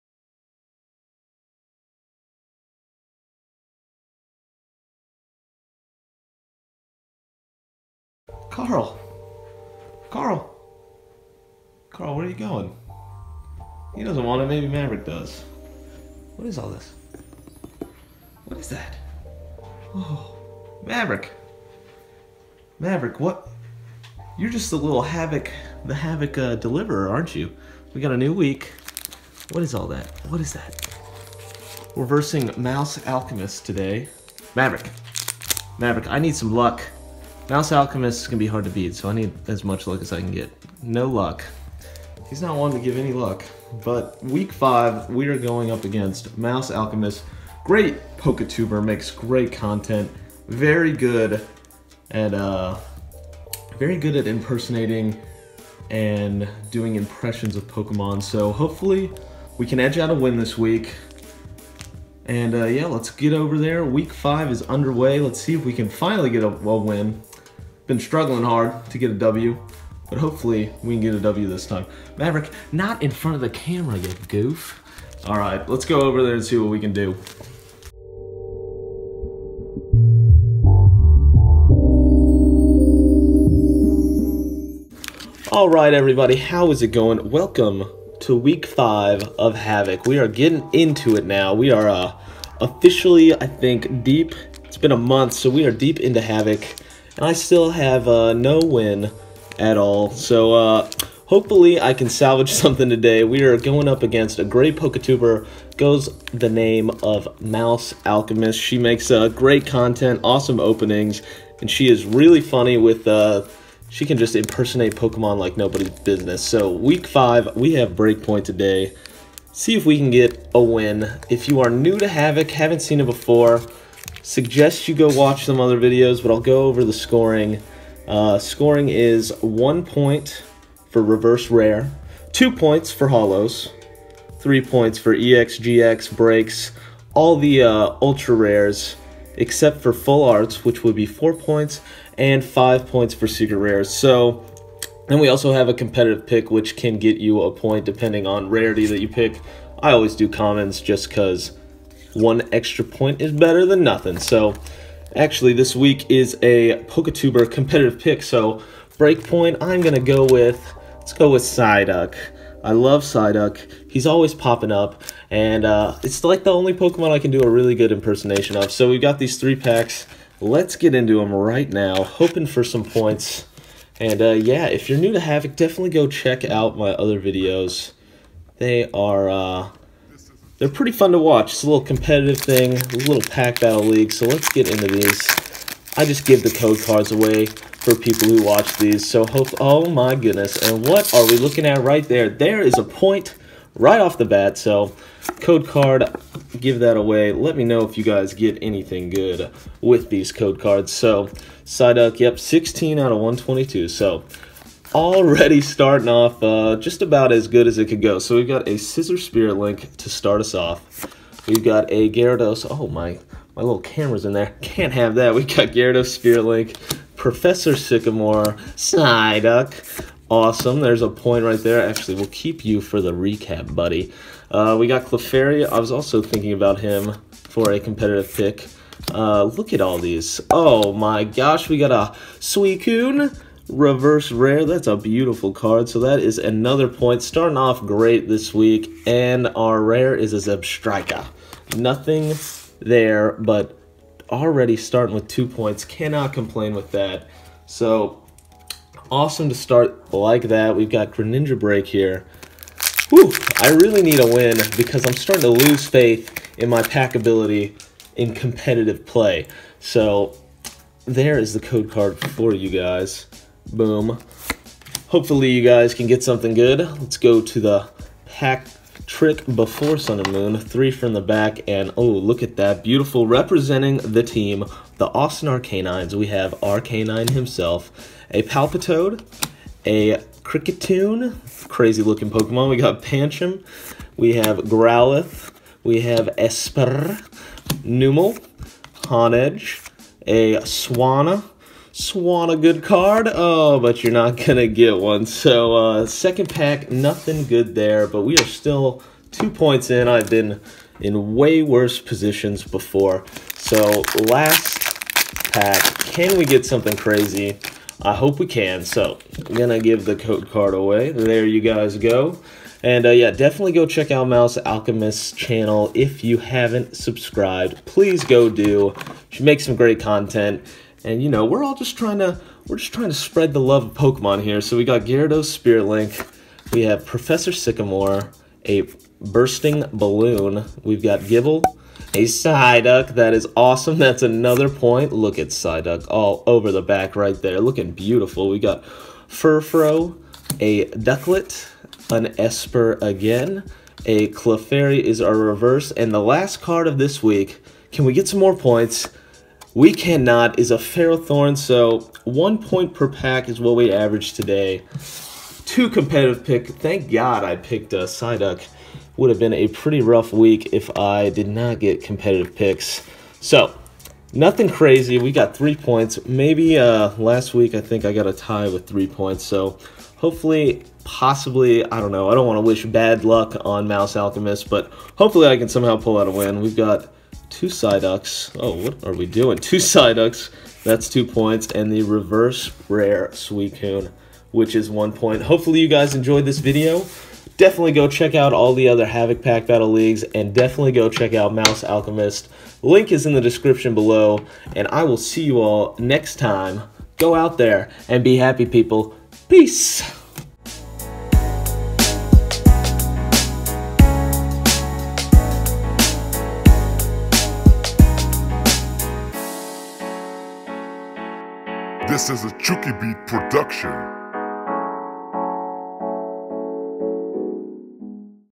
Pokemon - Havoc PBL Week 5 vs Mousealchemist!
Hey every one I am super excited because todays video is another duel in the HAVOC pack battle league! Today we are facing the awesome poketuber Mousealchemist! Make sure to check out her part of this battle on her video
I also just wanted to say thank to all of the people involved in this season of HAVOC and a huge shout out for helping everyone out by providing the packs for this season!